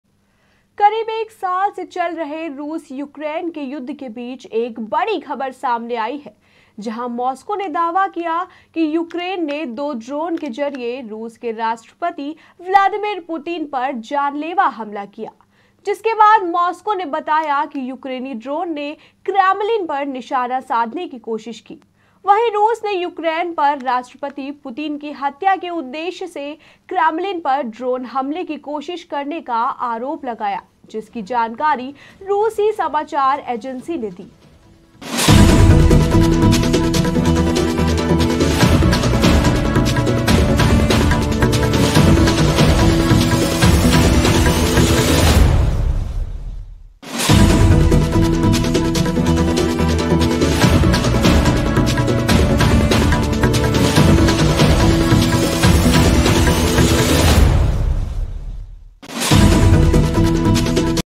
ड्रोन से पुतिन पर हमले की कोशिश, जानिए क्या है मामला ?
ड्रोन से पुतिन पर हमले की कोशिश, जानिए क्या है मामला ? aajkikhabar putin putinvladimir russiaukrainewar russiaukraineconflict latest news